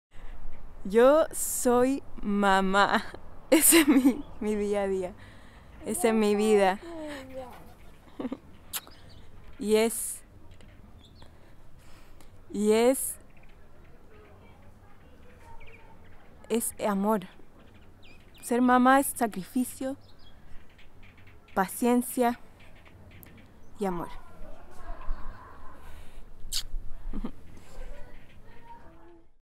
Soy MAMÁ...
¿Quiénes somos? es una serie de micro relatos generados de manera espontánea, con la honestidad de quien es sorprendido en la calle ante una pregunta: ¿Quién eres?

Un experimento audiovisual que, gracias a la apertura de los entrevistados, hoy se convierte en un espacio de búsqueda, reflexión y debate acerca de los elementos que nos definen como personas.